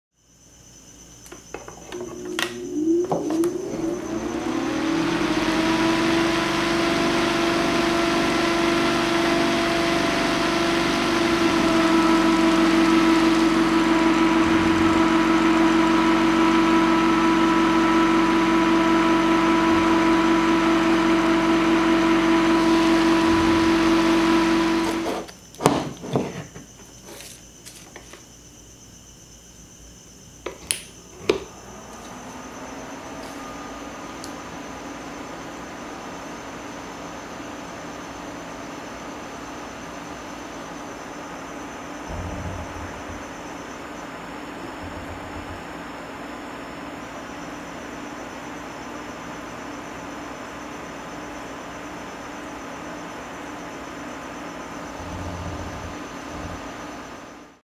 Noise Levels of a single 220V AC Fan vs 4 × 12V DC Fans
Noise levels of a single Proftech FAN-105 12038A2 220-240V AC 50/60Hz 0.14A vs SilverStone FN81 12V DC .011A × 4.